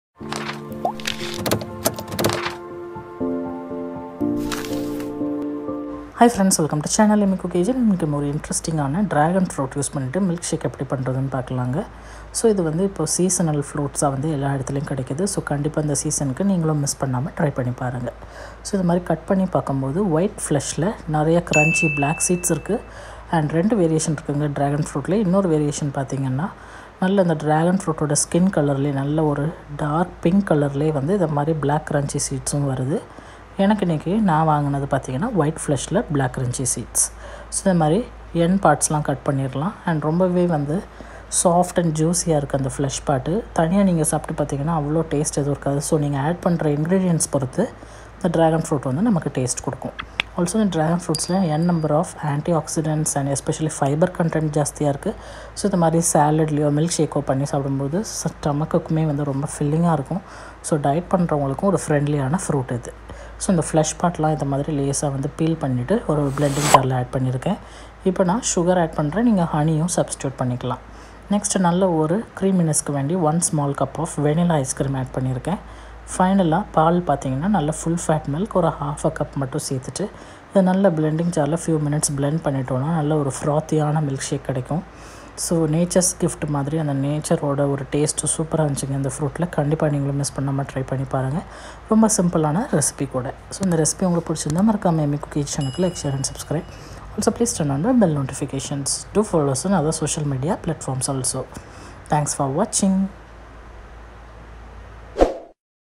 Dragon Fruit Milkshake in Tamil | Dragon Fruit Benefits | Seasonal Fruits
Hi Friend's in today's video,I have made simple,quick and delicious milkshake which is super easy to make.Many seasonal fruits are available and making use of them is a wise act.Dragon fruit comes in 2 variations one in pink flesh with crunchy black seeds & other with white flesh & black crunchy seeds.Dragon fruits are packed with fibre & anti-oxidants and super packed with nature's taste.Do try this milkshake and enjoy with family!!

**Ingredients**
Dragon fruit - 1
Milk-1/2 cup chilled
Vanilla ice cream-1 small tub
Sugar-2 tbsp